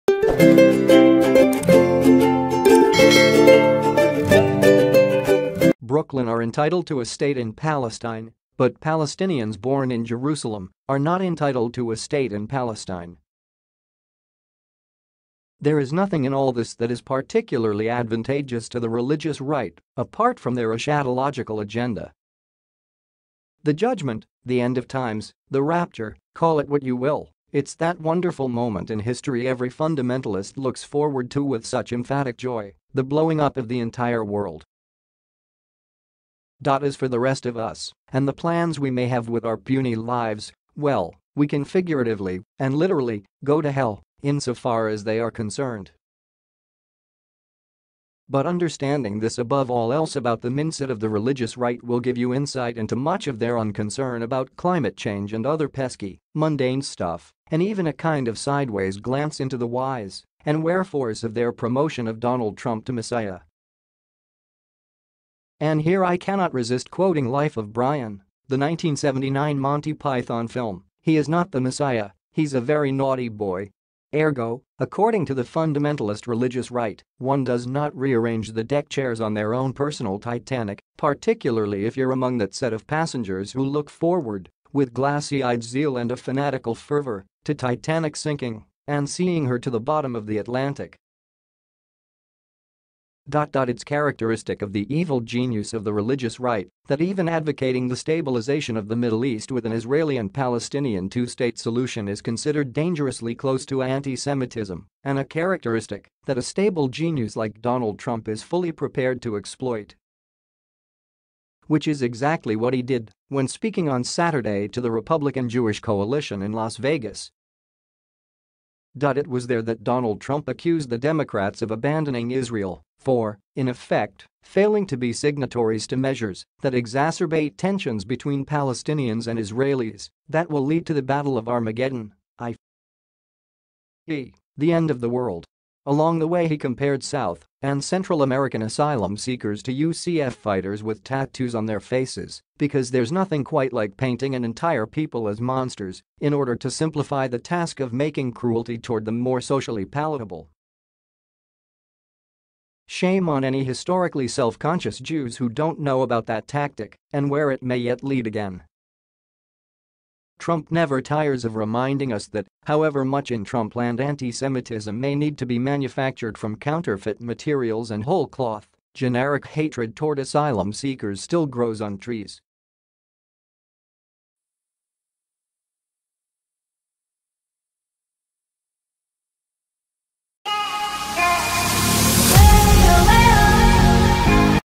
President Trump plays the anti-semitism card
Brooklyn are entitled to a state in Palestine, but Palestinians born in Jerusalem are not entitled to a state in Palestine.  There is nothing in all this that is particularly advantageous to the Relig